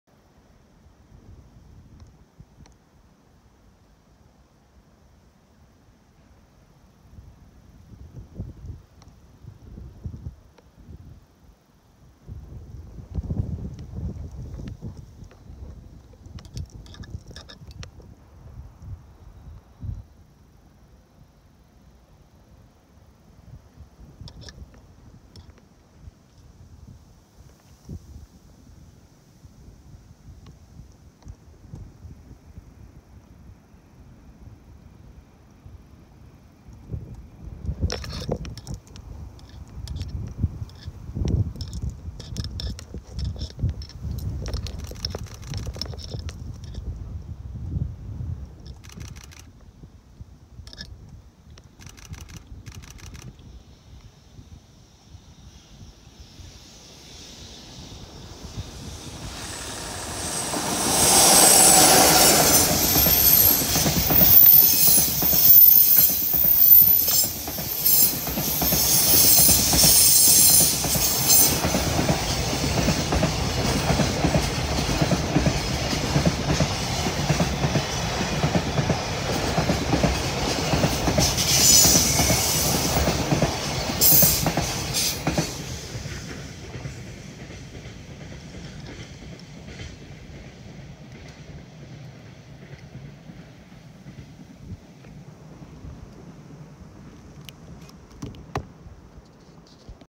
西濃カンガルーライナーSS60編成に代走で牽引したゼロロクニーナ
2018/11/10 東海道本線 醒ヶ井〜米原
EF66-27+コキ100形 西濃カンガルーライナーSS60 2059レ